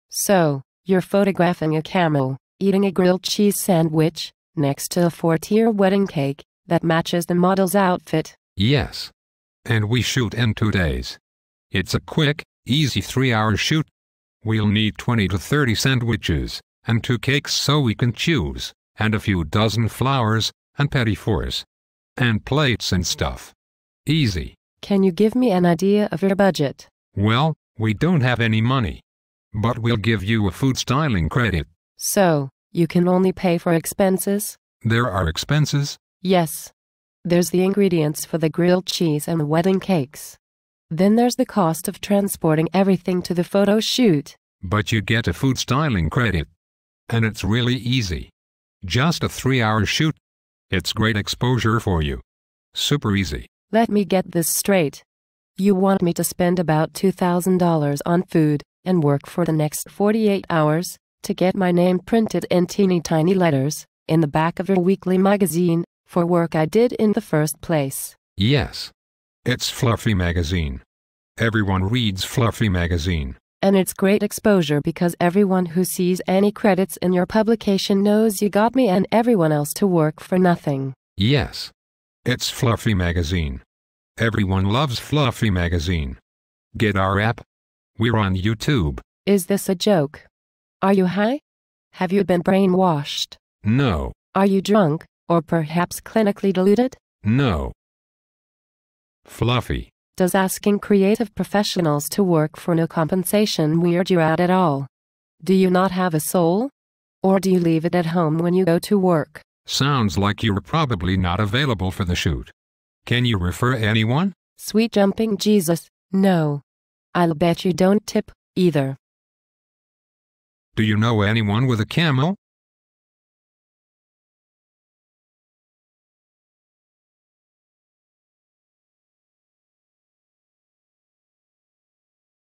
When food stylists daydream...
Loosely-based on actual real-life conversations in my line of work.
Slightly-embellished for your viewing pleasure.